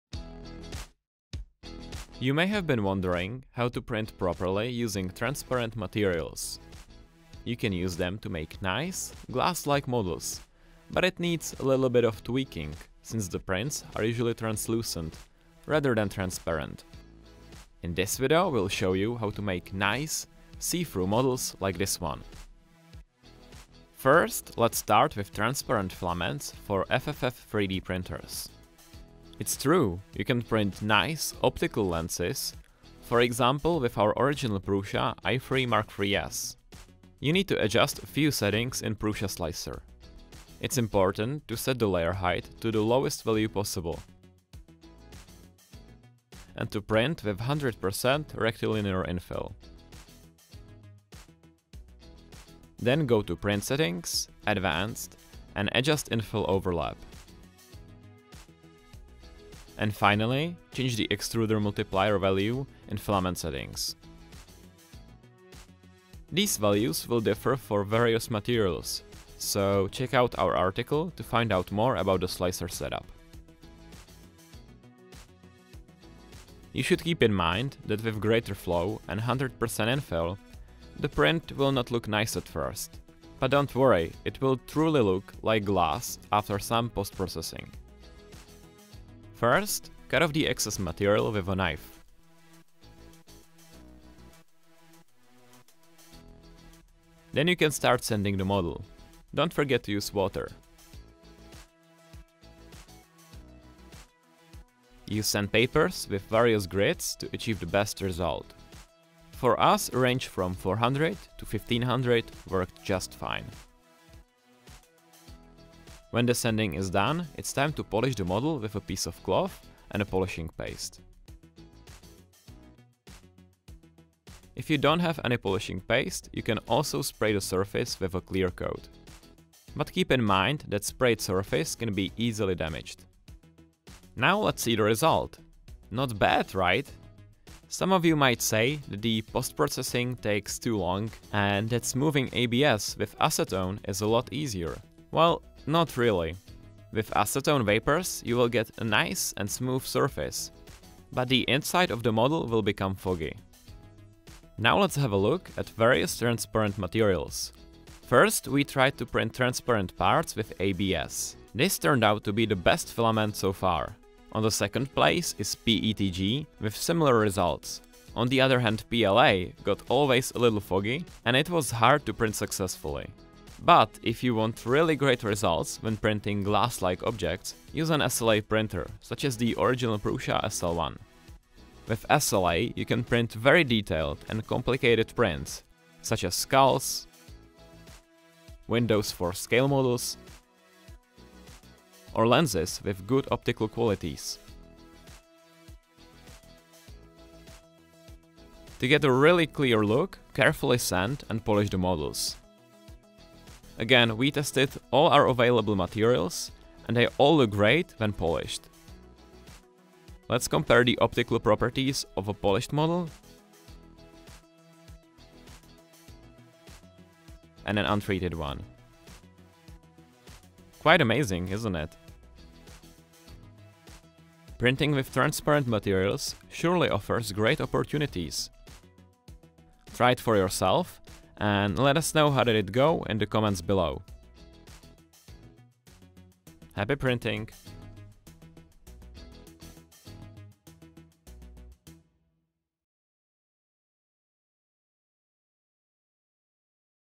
How to print with transparent materials
Did you know that you can print glass-like models with our Original Prusa MK3 and SL1 printers?